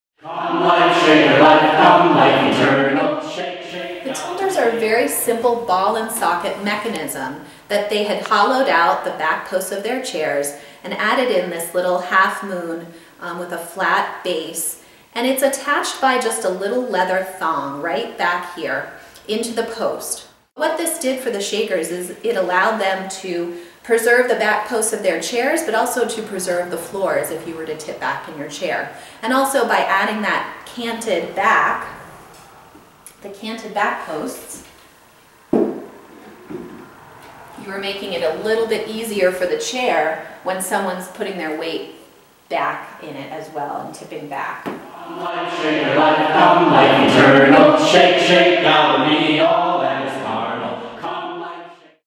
A Shaker innovation - the chair tilter
Not sure about you but mom would cuff me on the back of the head when she saw me leaning back on a chair.  She claimed I was being rude - but obviously the Shakers did not see it that way.  As a group they had an appreciation for joy and comfort.  As a result they invented this clever device to allow them to lean back and still protect the floors and feet of the chair.